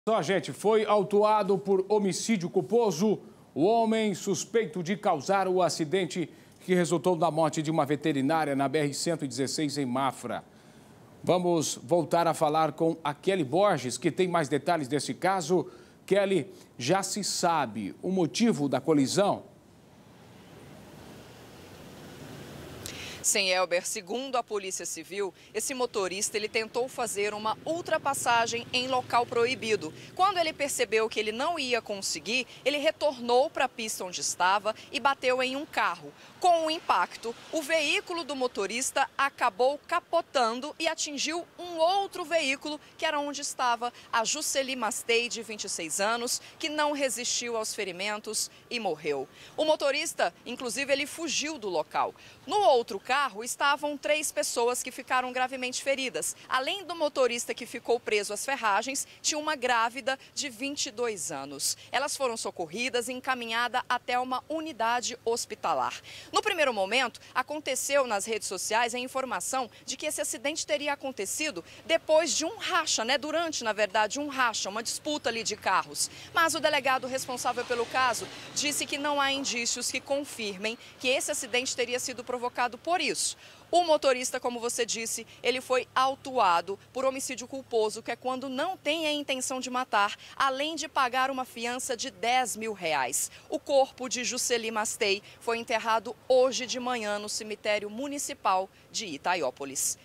Suspeito de causar acidente com morte na BR-116 é autuado por homicídio culposo
Foi autuado por homicídio culposo o homem suspeito de causar o acidente que resultou na morte de uma veterinária, na BR-116, em Mafra. Segundo a Polícia Civil, o motorista teria tentado realizar uma ultrapassagem em local proibido.